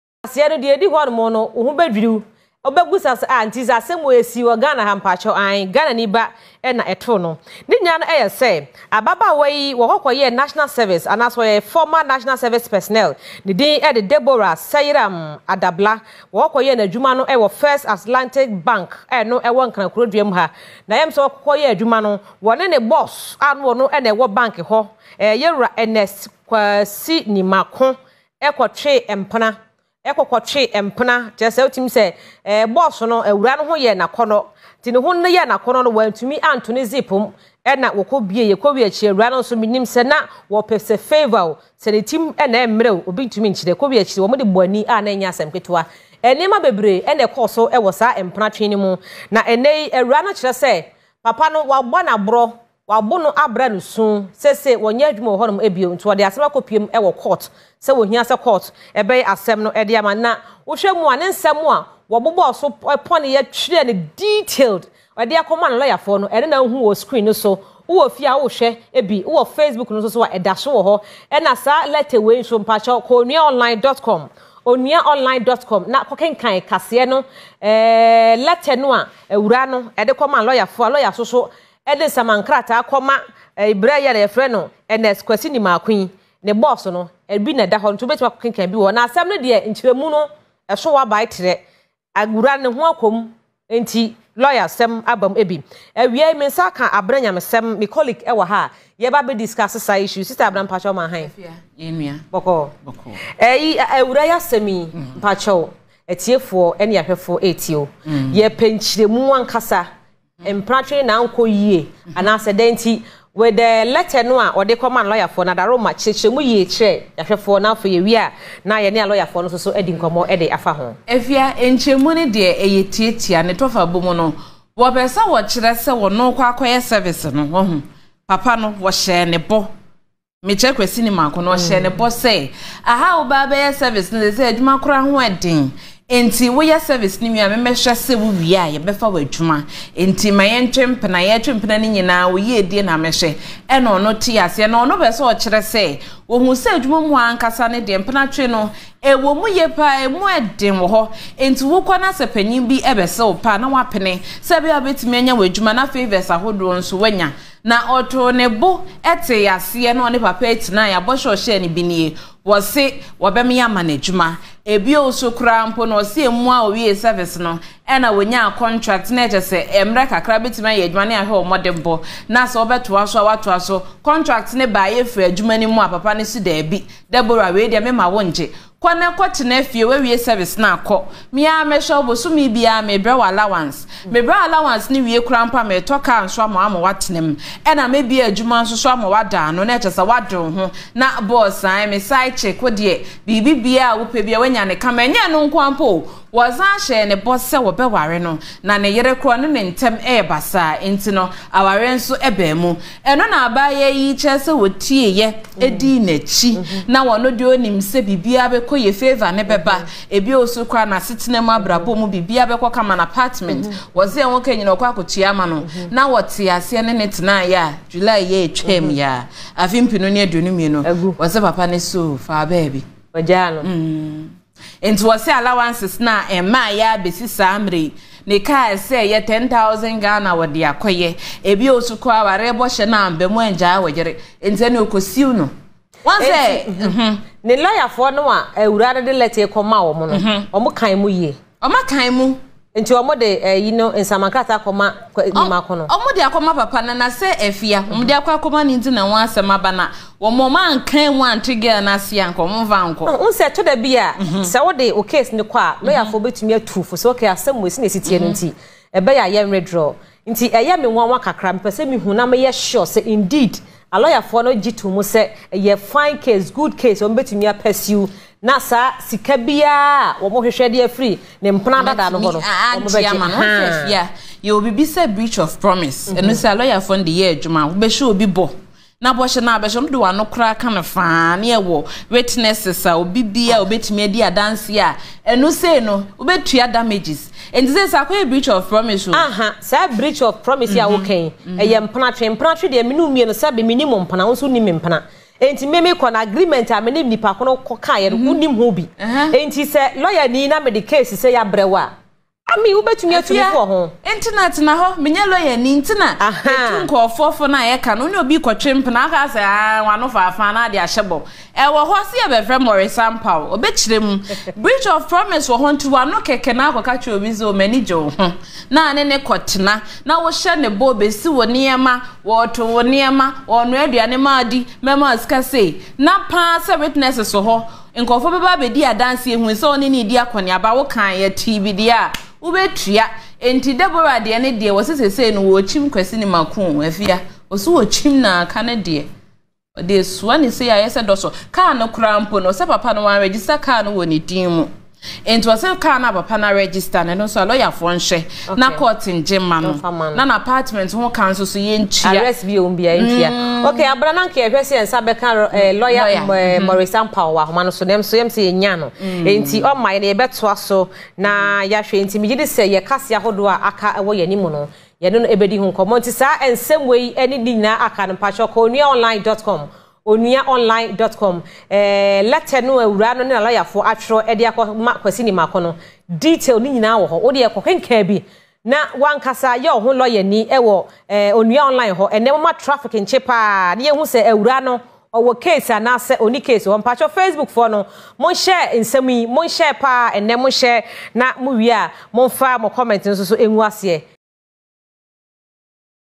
IS THIS A SET UP OR FOR REAL
A former NSS of First Atlantic Bank in Accra has filed a suit against one of the bank’s managers, Ernest Kwasi Nimako after the latter allegedly failed to uphold commitments made in their supposed intimate relationship.